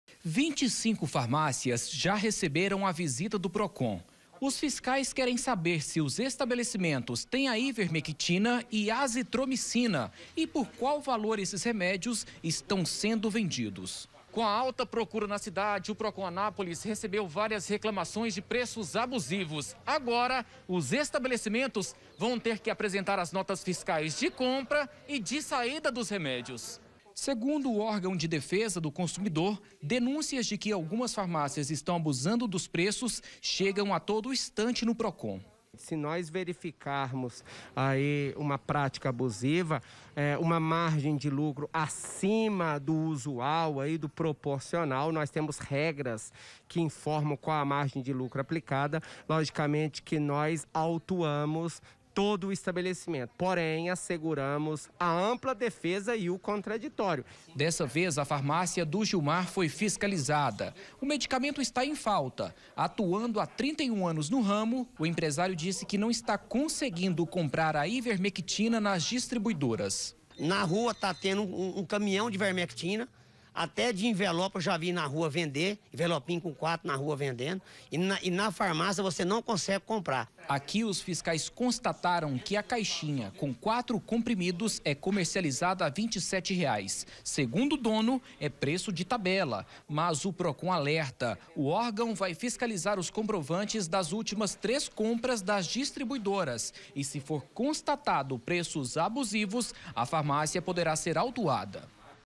PANDEMIA: PROCON FISCALIZA PREÇOS DE REMÉDIOS DO "KIT COVID"
O PROCON ESTÁ FISCALIZANDO OS PREÇOS DOS REMÉDIOS DO CHAMADO KIT COVID. MESMO SEM COMPROVAÇÃO CIENTÍFICA SOBRE A EFICÁCIA NO TRATAMENTO, A PROCURA AUMENTOU BASTANTE.